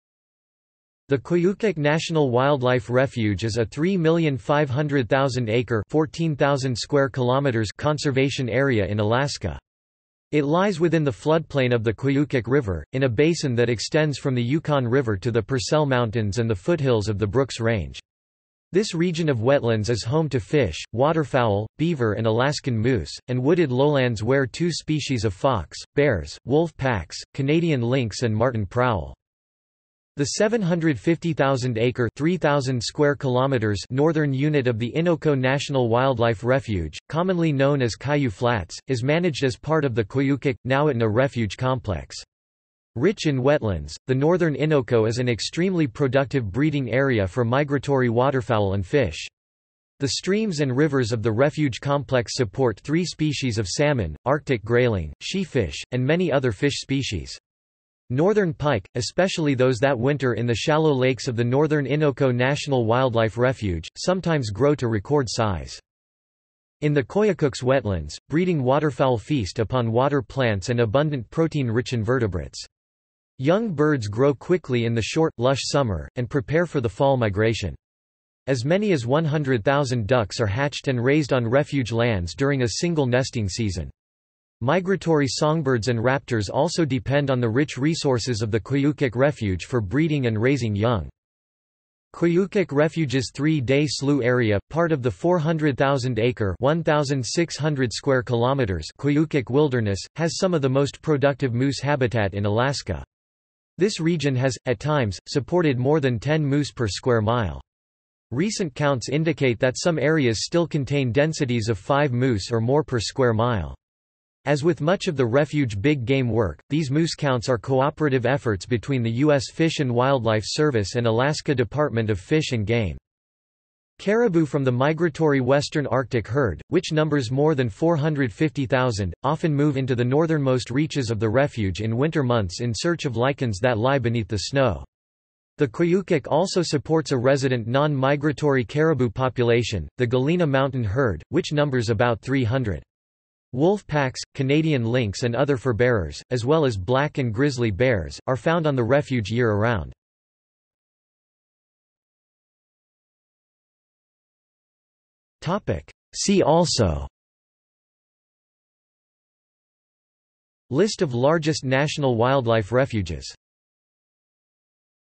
Koyukuk National Wildlife Refuge | Wikipedia audio article
This is an audio version of the Wikipedia Article:
Koyukuk National Wildlife Refuge

00:02:31 undefined

- Socrates

SUMMARY
The Koyukuk National Wildlife Refuge is a 3,500,000-acre (14,000 km2) conservation area in Alaska. It lies within the floodplain of the Koyukuk River, in a basin that extends from the Yukon River to the Purcell Mountains and the foothills of the Brooks Range. This region of wetlands is home to fish, waterfowl, beaver and Alaskan moose, and wooded lowlands where two species of fox, bears, wolf packs, Canadian lynx and marten prowl.
The 750,000-acre (3,000 km2) Northern Unit of the Innoko National Wildlife Refuge, commonly known as Kaiyuh Flats, is managed as part of the Koyukuk/Nowitna Refuge Complex. Rich in wetlands, the Northern Innoko is an extremely productive breeding area for migratory waterfowl and fish. The streams and rivers of the refuge complex support three species of salmon, Arctic grayling, sheefish, and many other fish species. Northern pike, especially those that winter in the shallow lakes of the Northern Innoko National Wildlife Refuge, sometimes grow to record size.
In the Koyukuk's wetlands, breeding waterfowl feast upon water plants and abundant protein-rich invertebrates. Young birds grow quickly in the short, lush summer, and prepare for the fall migration. As many as 100,000 ducks are hatched and raised on refuge lands during a single nesting season. Migratory songbirds and raptors also depend on the rich resources of the Koyukuk Refuge for breeding and raising young.
Koyukuk Refuge's Three-Day Slough area, part of the 400,000-acre (1,600 km2) Koyukuk Wilderness, has some of the most productive moose habitat in Alaska. This region has, at times, supported more than 10 moose per square mile. Recent counts indicate that some areas still contain densities of five moose or more per square mile. As with much of the refuge big game work, these moose counts are cooperative efforts between the U.S. Fish and Wildlife Service and Alaska Department of Fish and Game.
Caribou from the migratory Western Arctic Herd, which numbers more than 450,000, often move into the northernmost reaches of the refuge in winter months in search of lichens that lie beneath the snow. The Koyukuk also supports a resident non-migratory caribou population, the Galena Mountain Herd, which numbers about 300. Wolf packs, Canadian lynx and other furbearers, as well as black and grizzly bears, are found on the refuge year around.